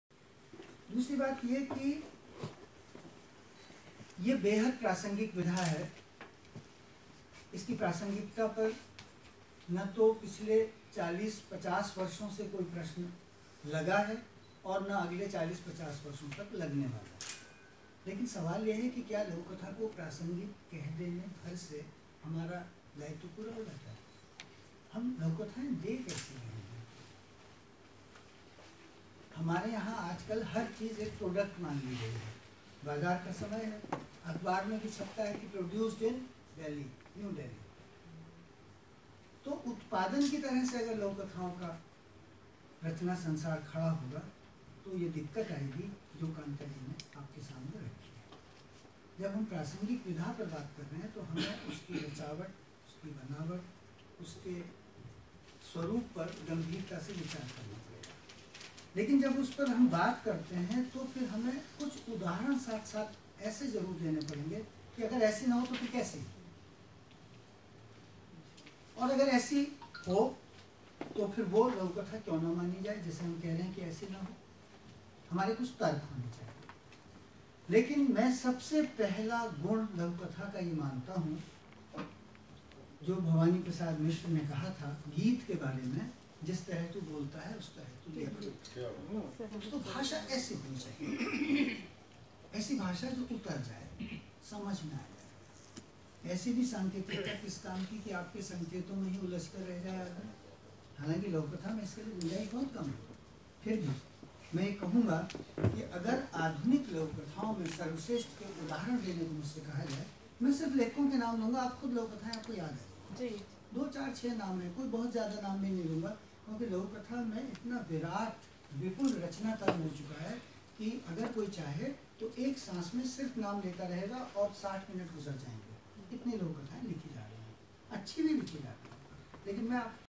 लघुकथा एक प्रासंगिक विधा है // महेश दर्पण
नयी दिल्ली-परिचय साहित्य परिषद् द्वारा आयोजित लघुकथा-पाठ एवं चर्चा-गोष्ठी में ~ चित्रा मुद्गल,महेश दर्पण, सुभाष चंदर, सुभाष नीरव,श्रीमती कांता राय (भोपाल), लक्ष्मी शंकर वाजपेयी ।।  *रचना-पाठ* डाॅ. सीतेश आलोक, अशोक वर्मा , हरनाम शर्मा ,घनश्याम मैथिल 'अमृत(भोपाल),  डाॅ. पूरन सिंह, मनोज अबोध, डाॅ. सुषमा गुप्ता, शोभा रस्तोगी,श्रीमती उपमा, नीता सैनी,अंजू खरबन्दा,सन्दीप तोमर,अलका सिन्हा ।  रशियन विज्ञान एवं संस्कृति केन्द्र, नयी दिल्ली । इस अवसर पर अनिल 'मीत'  सुभाष नीरव   लक्ष्मी शंकर वाजपेई  उर्मिल भूषण की उपस्तिथि उल्लेखनीय रही।।